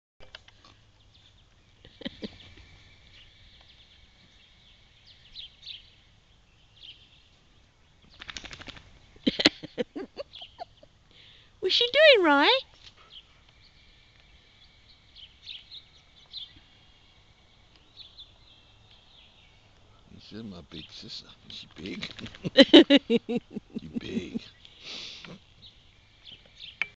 Sasha and Riley. June 2009
Female Boxer Sasha rolling in the grass, she's a happy girl :) Our ShihTzu/Lhasa mix boy Riley is checking her out!